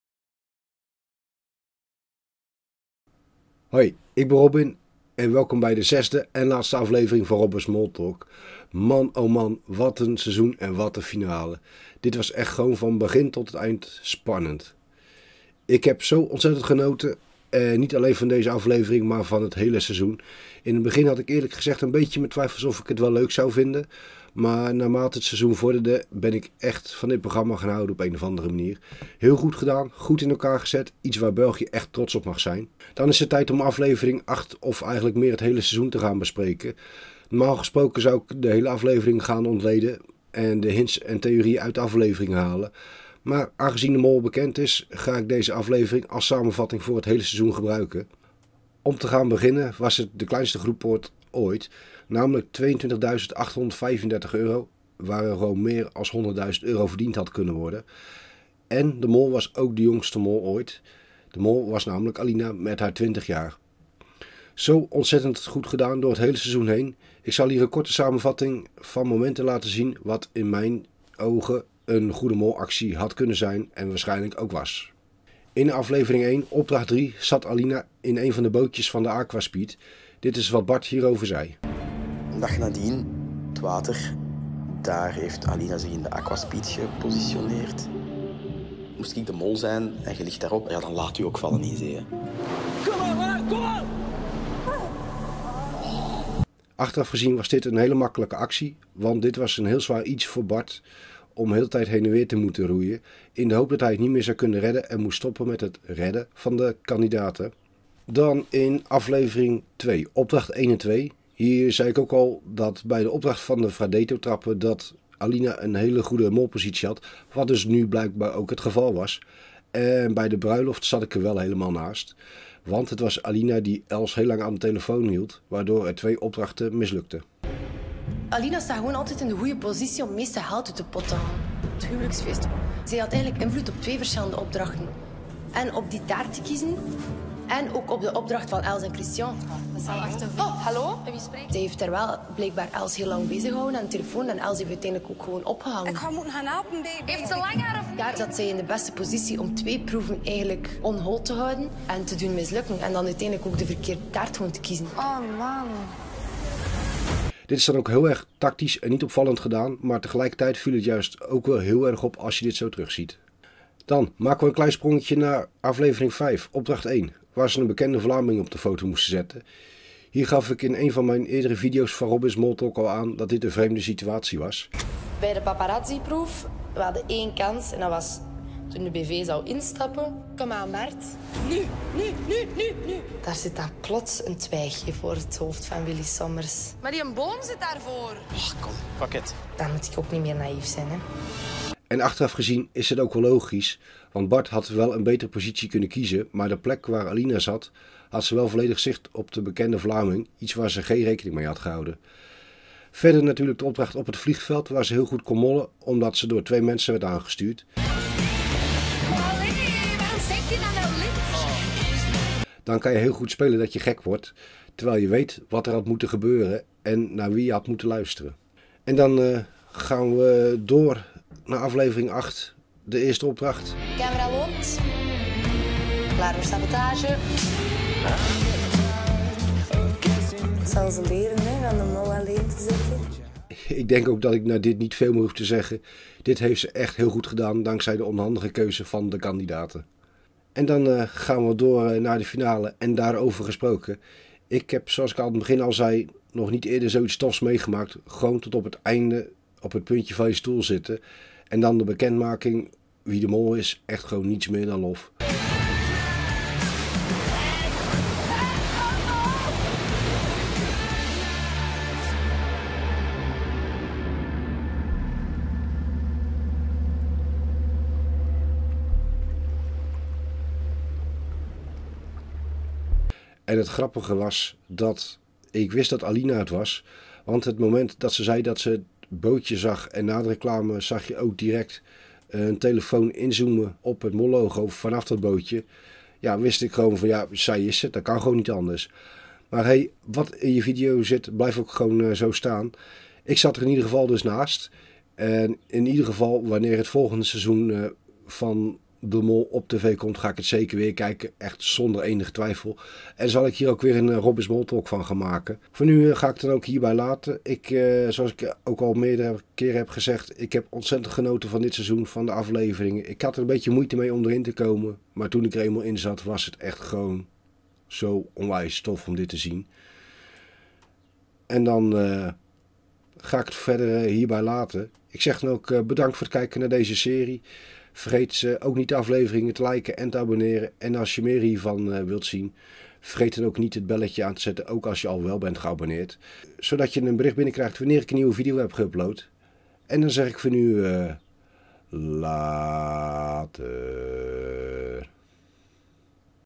De mol is bekend na een spannende finale! || Robin's Moltalk - De Mol België Hints en Theorieën #8
Welkom bij de zesde en laatste aflevering van Robin's Moltalk! Dit is een serie welke ik wekelijks heb geupload met alle hints en theorieën van De Mol 2020. En vandaag is de mol ontmaskerd!

Heel veel kijkplezier toegewenst!